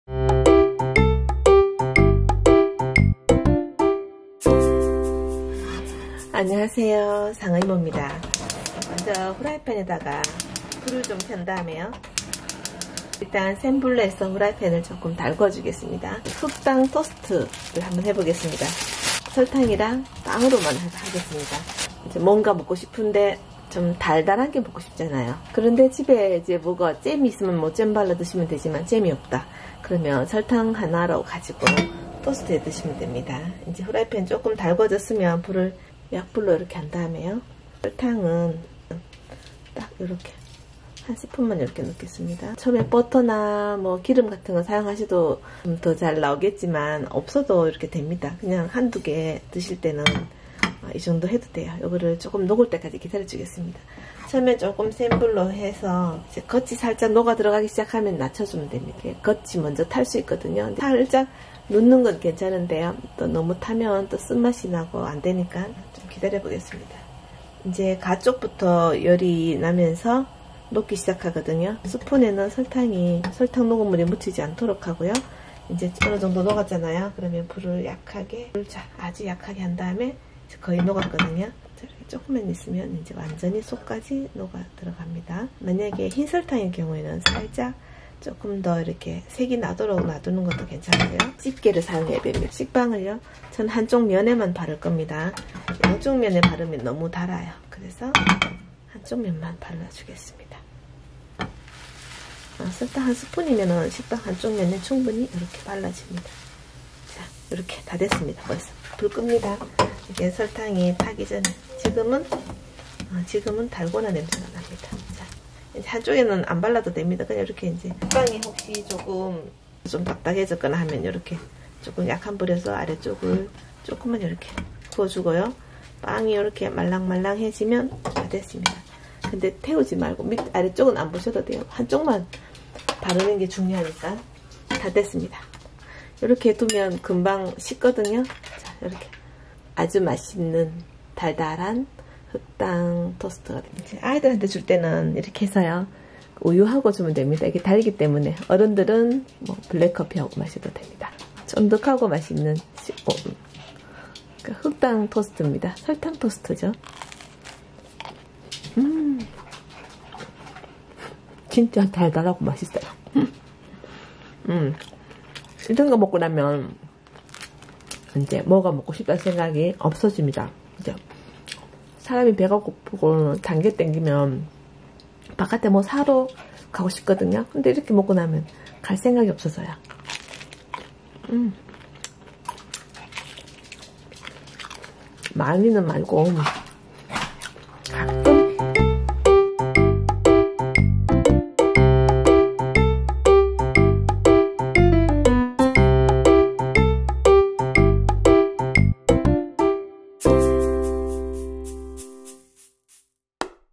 흑당토스트/설탕토스트 빵한조각남았길래 설탕토스트해먹었더니 너무 맛나요 !! [상어이모.SANGEOIMO]
흑당토스트만들기
한장남아서 돌아다니는 식빵은 이렇게 해결!!